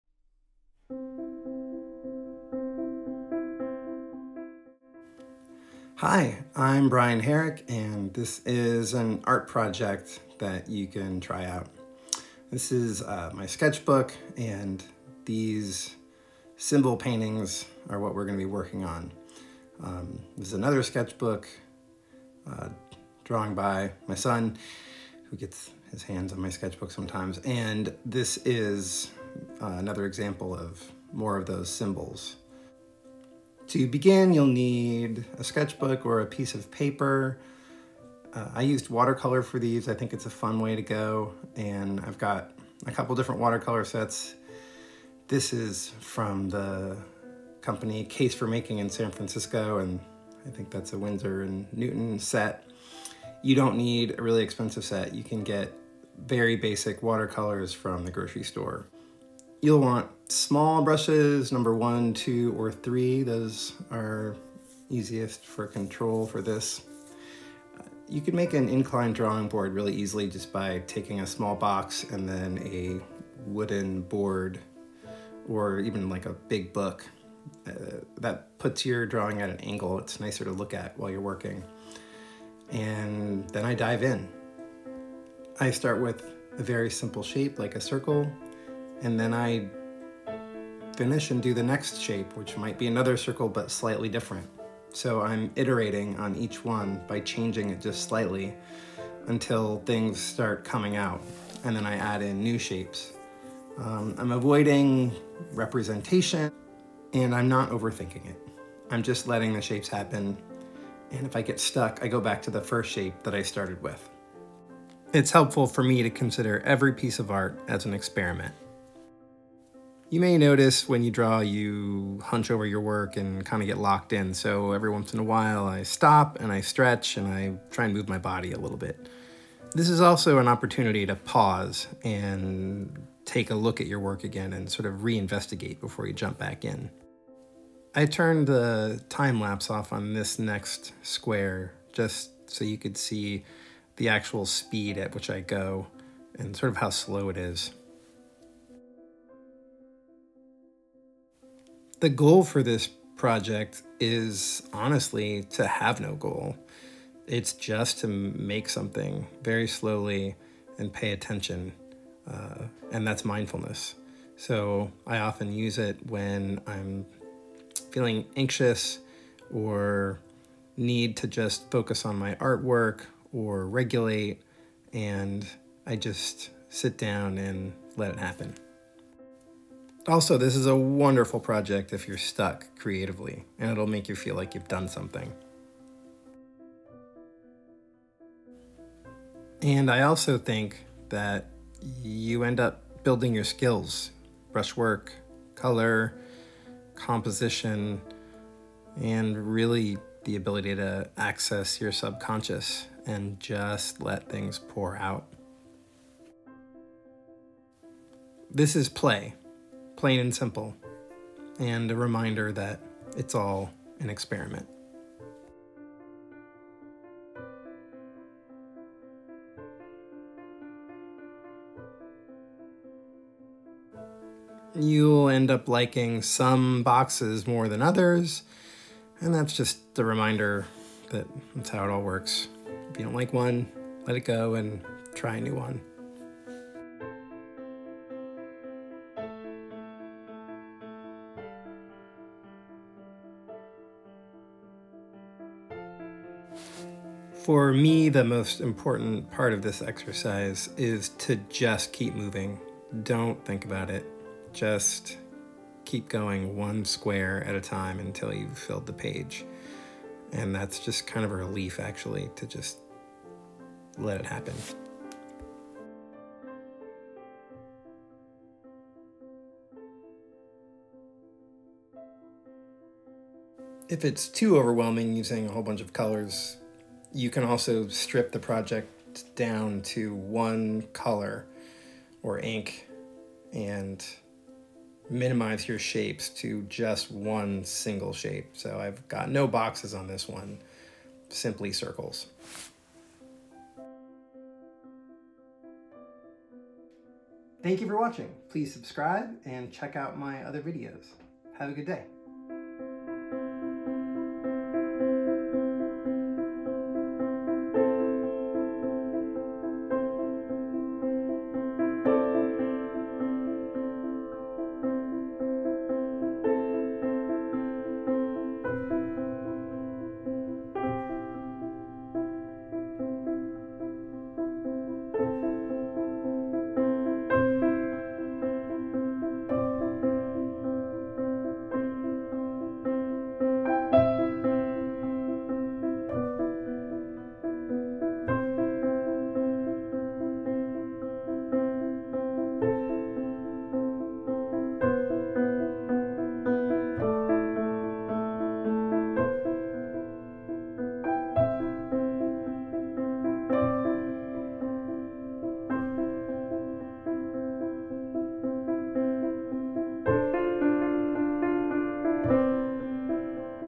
Symbol Painting / Soothing & Relaxing Demo / Watercolor Painting / Mindful Art / Daily Art Therapy
Create a simple page of symbols for the sake of mindfulness, relaxation, building art skills, and having fun!  Original music by Stefan Nelson at Haptic Realm